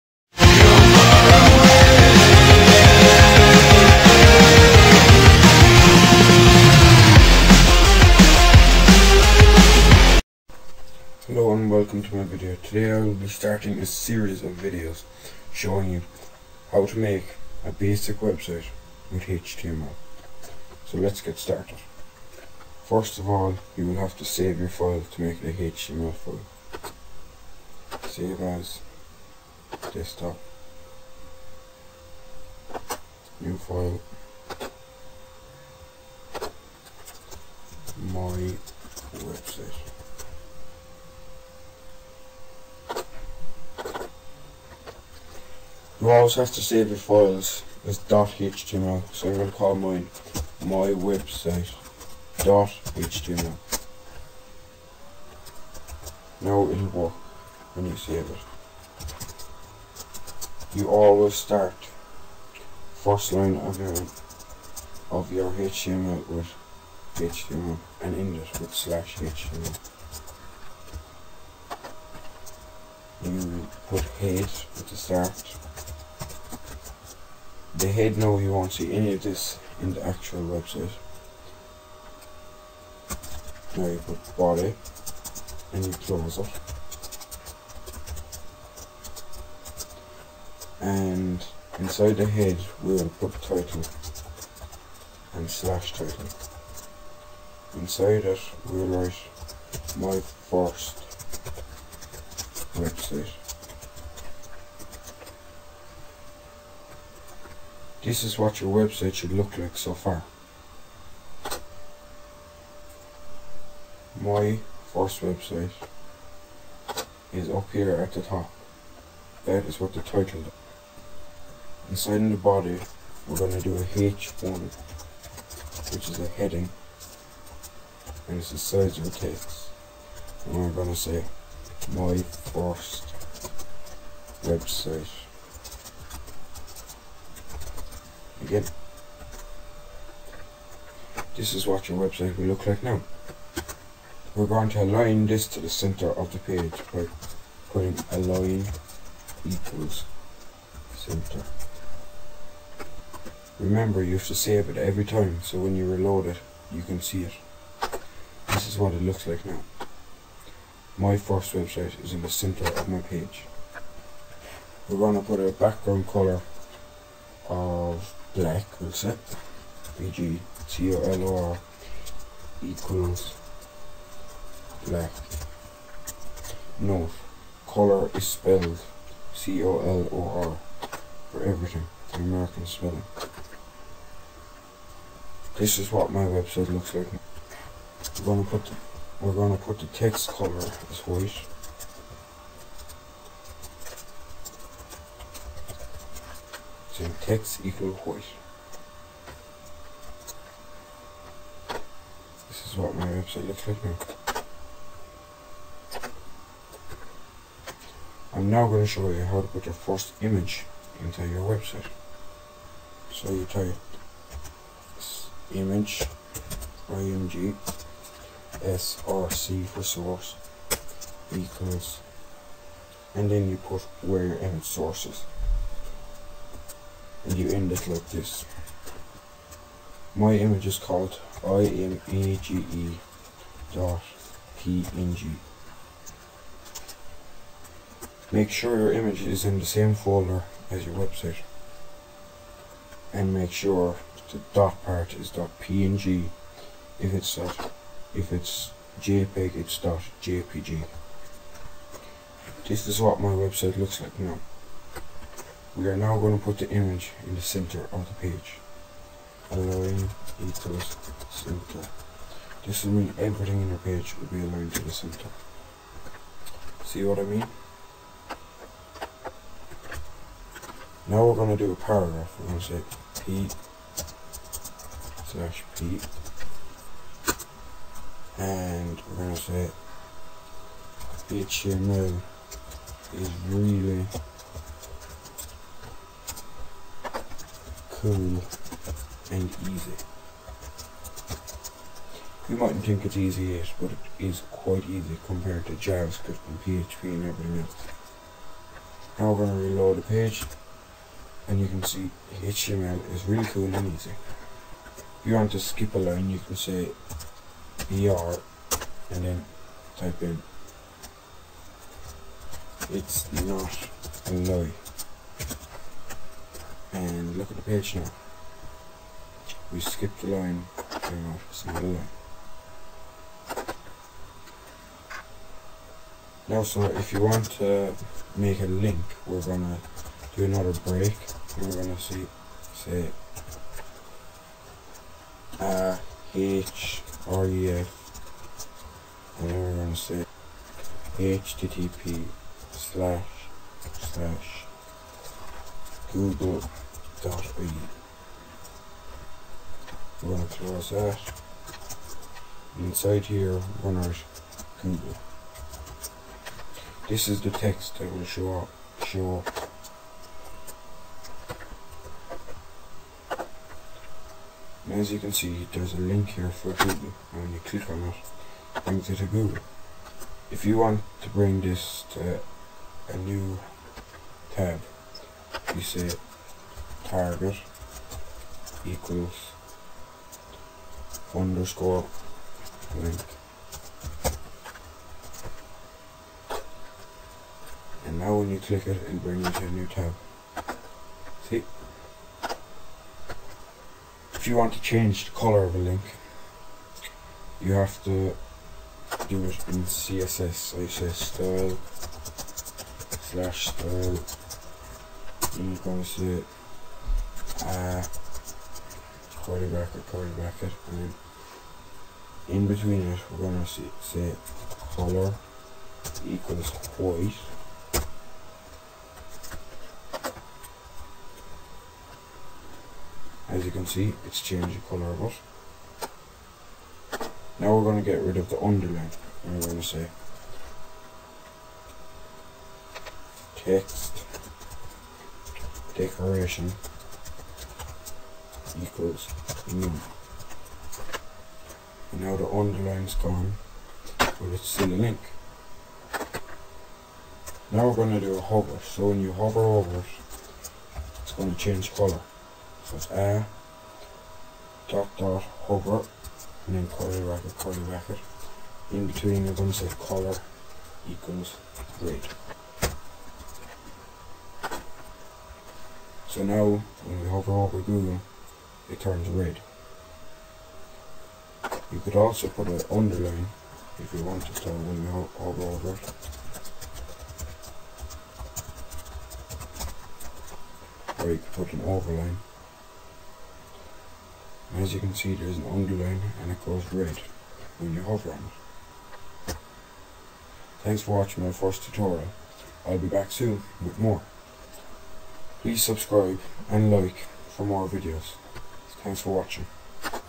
Learn HTML Tutorial 1
Some CSS
How To Make A Link
How To Put An Image In